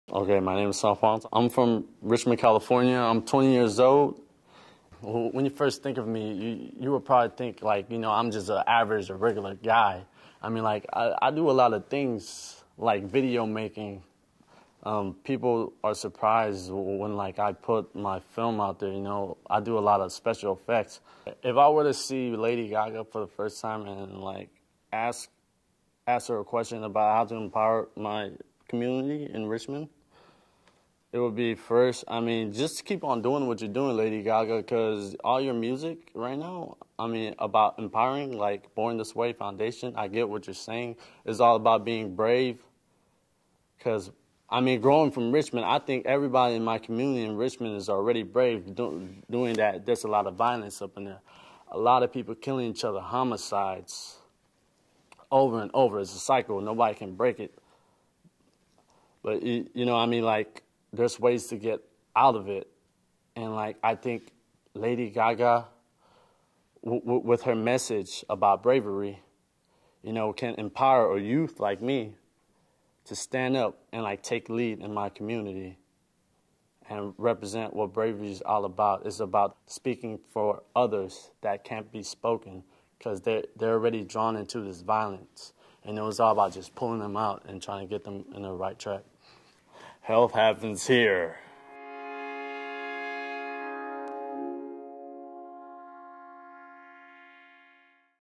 Born Brave - Sompong Vieng Vilai, Richmond BHC, CA
Sompong Vieng Vilai, age 20, from Richmond Building Healthy Communities, CA is one of the 18 youth leaders The California Endowment selected from communities across the state to take part in the official launch of Lady Gaga's Born This Way Foundation (BTWF). BTWF focuses on empowering youth to be leaders and advocates in their communities.

Visit the Born Brave Tumblr site for more youth delegate stories